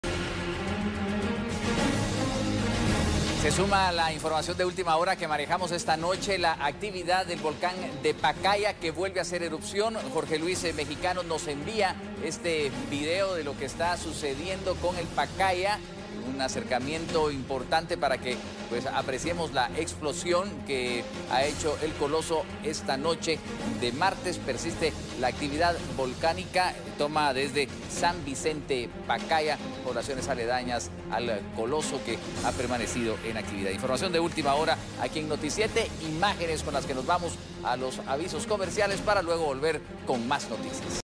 Persiste la actividad volcánica en el Pacaya
Se registró una fuerte explosión

Persiste la actividad volcánica del Pacaya, el cual ha hecho erupción durante la noche de este martes, según el reporte de los vecinos.